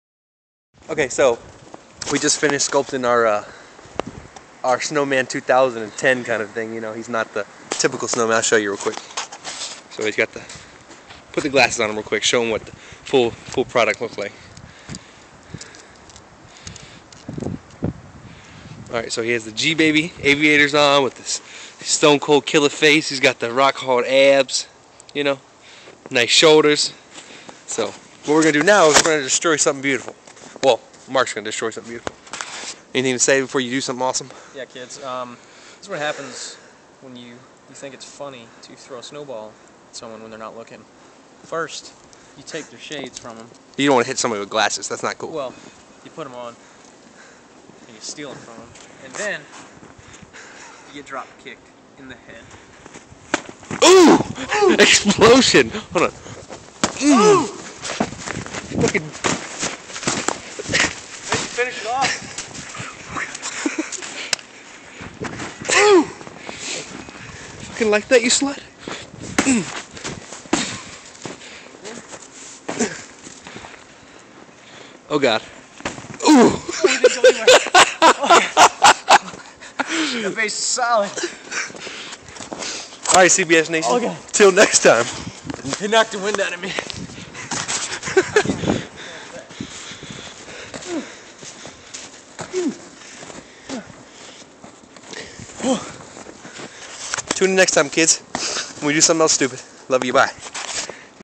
Greatest Snowman of ALL TIME!!
Had a big snow storm here in VA finally.  Had a little fun!

Watch the END!  THE Base is solid ice and when he tackles it, it doesnt MOVE!!! HAHAHAHAH

tags ignore